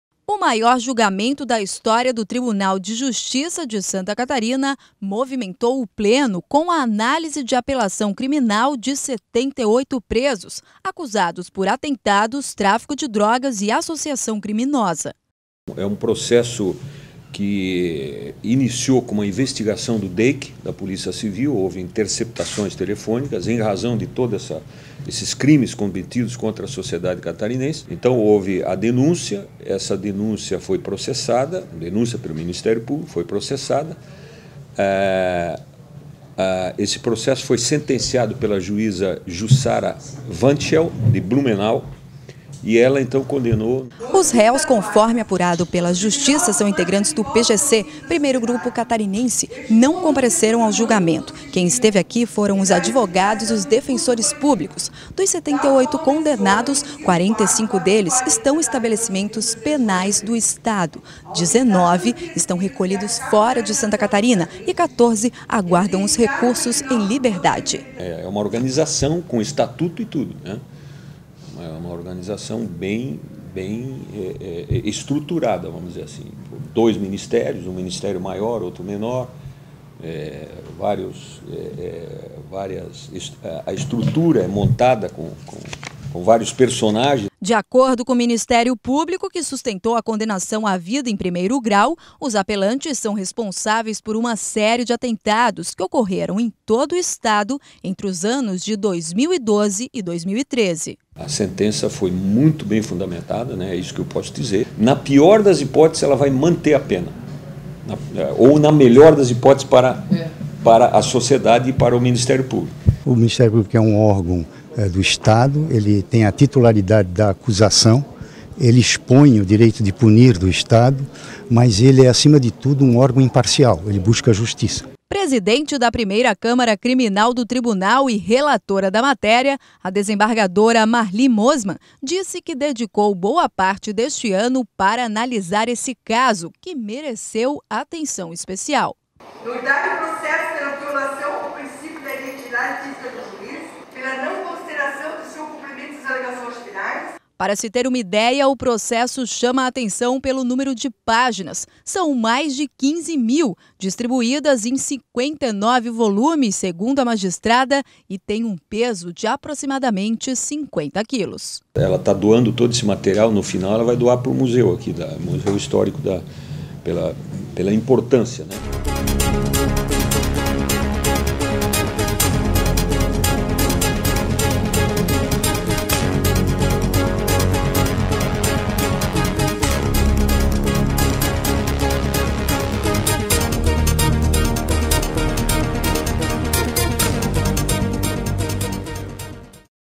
TJSC realiza o maior julgamento de sua história
A 1ª Câmara Criminal do Tribunal de Justiça concluiu nesta tarde (4/12), às 14h30min, o maior julgamento da história da instituição e confirmou na íntegra a sentença da comarca de Blumenau que condenou 80 réus integrantes da organização criminosa Primeiro Grupo Catarinense (PGC) a penas que, somadas, atingiram 1.031 anos.

O órgão julgador não promoveu qualquer alteração na decisão de 1º grau ao analisar apelações interpostas por 78 condenados ¿ dois preferiram não recorrer ao TJ. "A sentença é irretocável", comentou a desembargadora Marli Mosimann, presidente da 1ª Câmara e relatora dos recursos. Ela fez questão de tornar público seu apreço pelo trabalho realizado pela juíza Jussara Schittler dos Santos Wandscheer, responsável pela condução do processo na comarca de origem.

O desembargador Paulo Roberto Sartorato seguiu a mesma posição da desembargadora Marli. "Diante do conjunto probatório robusto, com elementos suficientes para dar sustentação às condenações, não tenho dúvida em acompanhar o voto da relatora no sentido de manter a sentença em sua integralidade", pronunciou-se o desembargador Luiz Cesar Schweitzer, último integrante da câmara a dar seu voto e oficializar o entendimento unânime dos julgadores.

No auditório do TJ, local que abrigou a sessão de julgamento, poucas pessoas estiveram presentes para acompanhar os trabalhos, que se estenderam por mais de 10 horas nos últimos dois dias. Magistrados, procuradores, advogados, assessores, serventuários, alguns poucos acadêmicos de Direito e outros parentes dos réus, assim como representantes dos órgãos de imprensa e homens da segurança, formaram o público constante na condução dos trabalhos e na audiência.

O esquema de segurança montado pela Casa Militar do TJ funcionou muito bem, sem registro de qualquer alteração ao longo da sessão. Para ingressar no auditório, era obrigatória a apresentação de documento de identidade com foto, assim como submissão aos aparelhos detectores de metal.

Com o encerramento do julgamento, o acórdão segue para publicação no Diário da Justiça Eletrônico nos próximos dias, com abertura de prazo para eventuais novos recursos da defesa e possibilidade de se levar o caso para análise aos tribunais superiores em Brasília (Ap. Crim. n. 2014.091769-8).